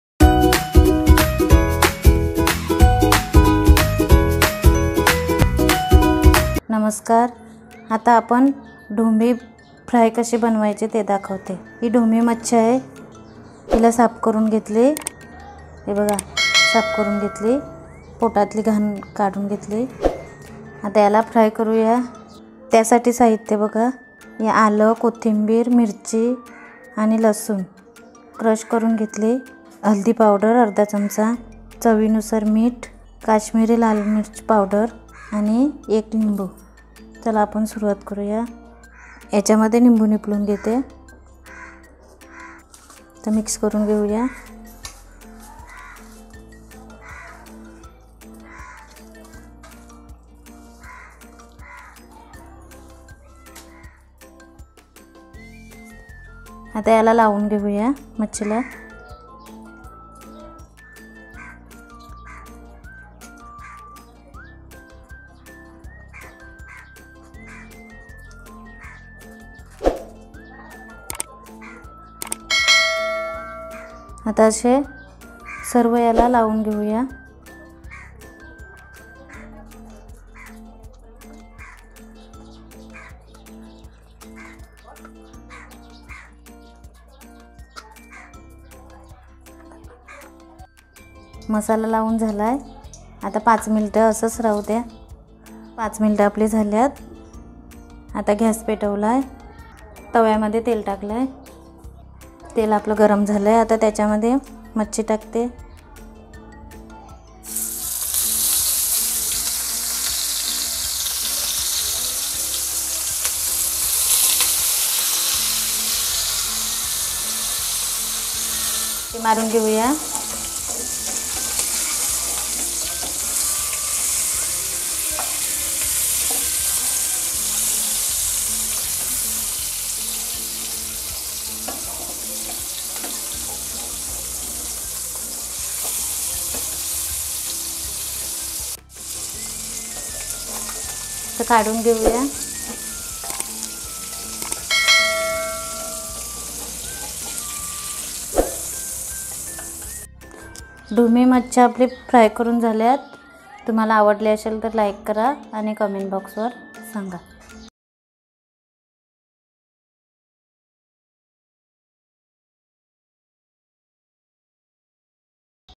ढोमी फिश फ्राय | Silver Croaker (Dhomi) Fish Fry | Koli Taste
Ingredients:
Dhomi fish, Grinded paste(Ginger, Garlic, Green chillies and Coriander leaves), ½tbsp Turmeric powder, 1tbsp Kashmiri red chili powder, 1 Lemon, Salt as per taste, Oil.

Steps:
1. Clean the Dhomi fish properly and make slits on the fish.
2. Take a vessel, add the Grinded paste, Turmeric powder, Kashmiri red chili powder and salt together.
3. Squeeze a lemon in the marination and mix it properly.
4. Apply the prepared marination on the fish properly.
5. Let the fish marinate for 5 mins.
6. Take oil in a pan and fry the fish till it gets crispy and golden brown.
7. Garnish it with coriander leaves and lemon.
Your dish is ready!